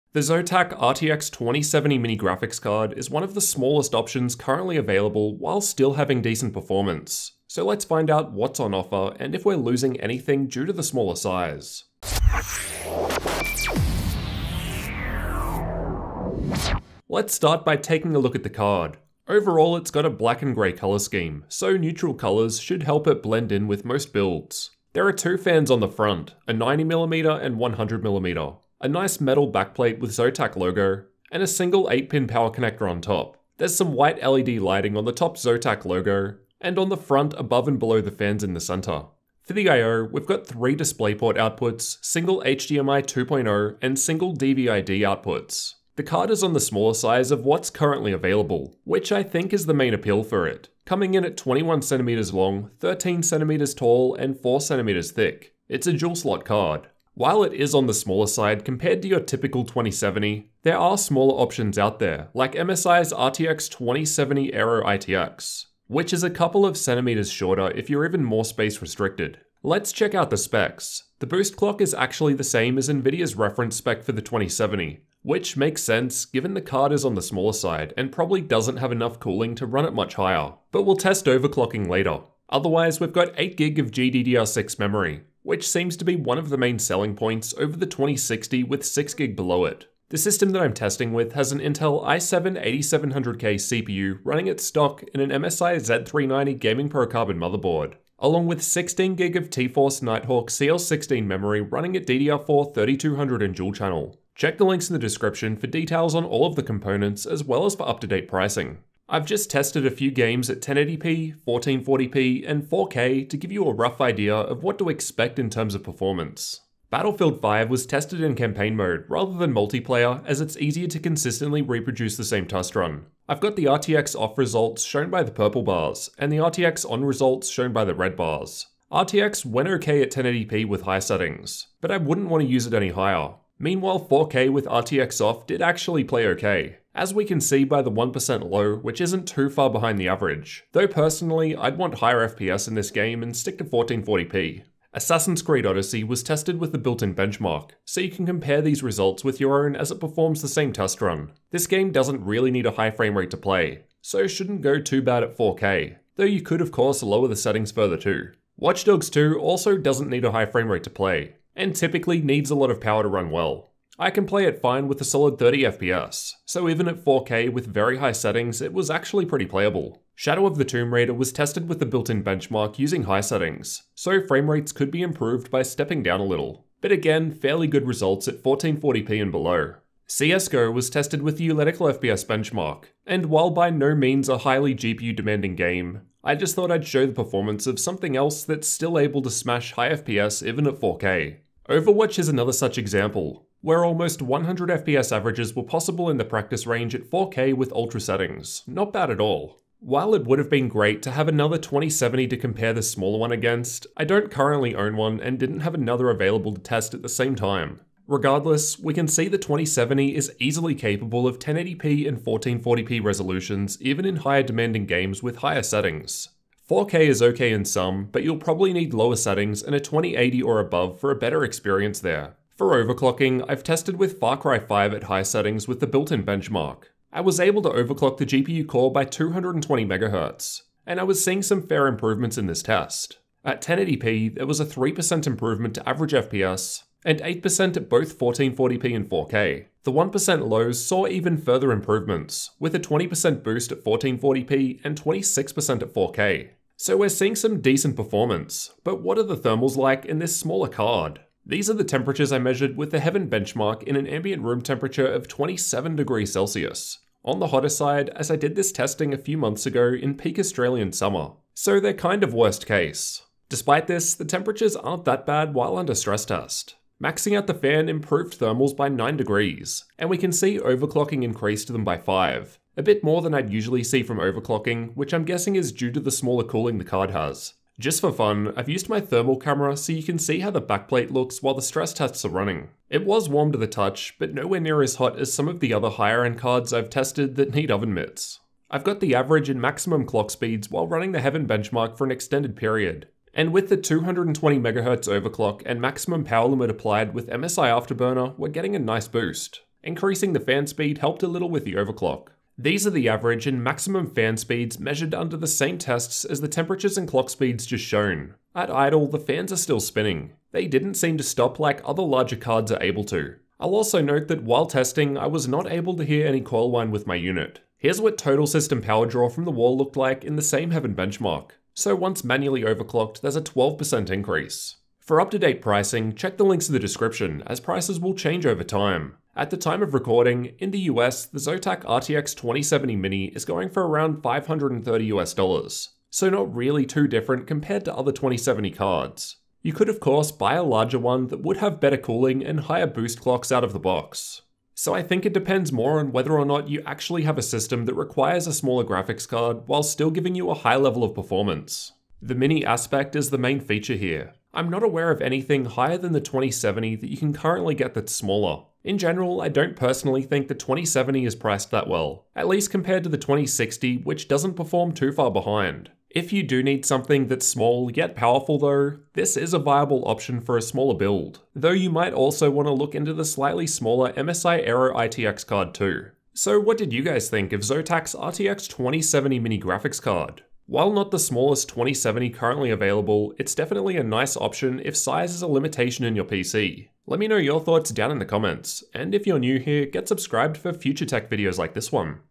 Zotac RTX 2070 Mini Review - Small and Powerful?
The Zotac RTX 2070 graphics card is one of the smallest options currently available while still having decent performance, so let’s find out what’s on offer and if we’re losing anything due to the smaller size. I’ll be testing gaming at 1080p, 1440p and 4K, as well as overclocking, cooling, power draw and fan speed to help you decide if it’s worth buying.

*** Check Zotac RTX 2070 Mini Prices at Amazon ***

Intel i7-8700K
TeamGroup Night Hawk RGB Memory
MSI MPG Z390 Gaming Pro Carbon Motherboard
NZXT H700 Case
Fractal S36 AIO Cooler